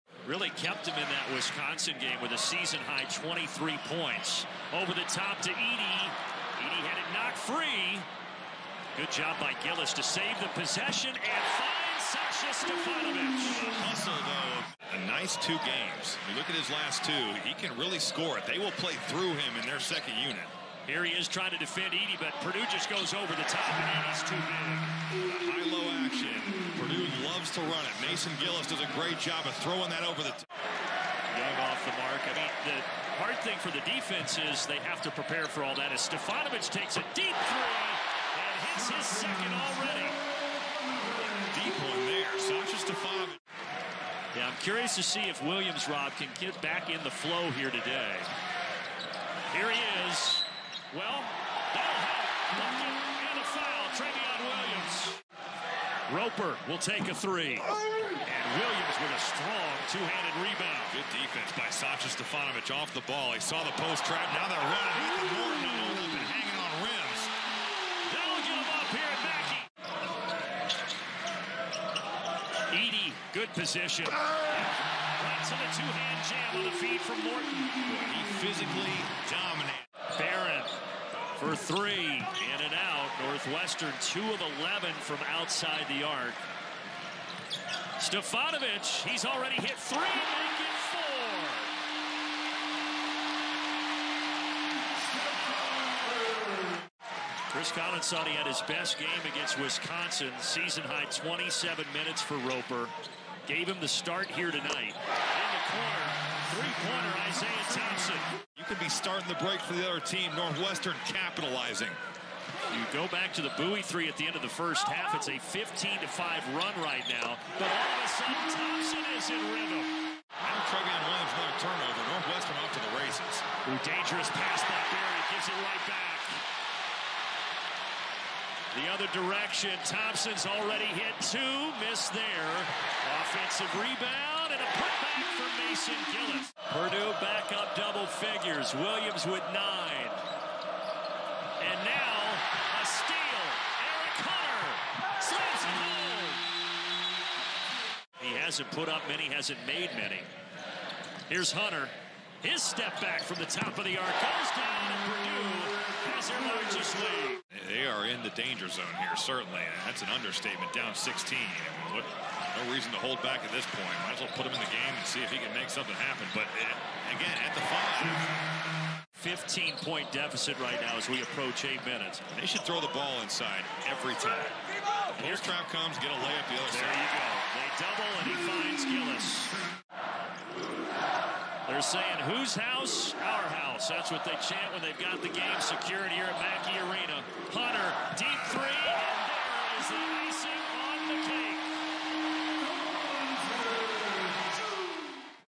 HIGHLIGHTS: No. 4 Purdue 80, Northwestern 60
No. 4 Purdue defeated Northwestern 80-60 at Mackey Arena on Jan. 23, 2022.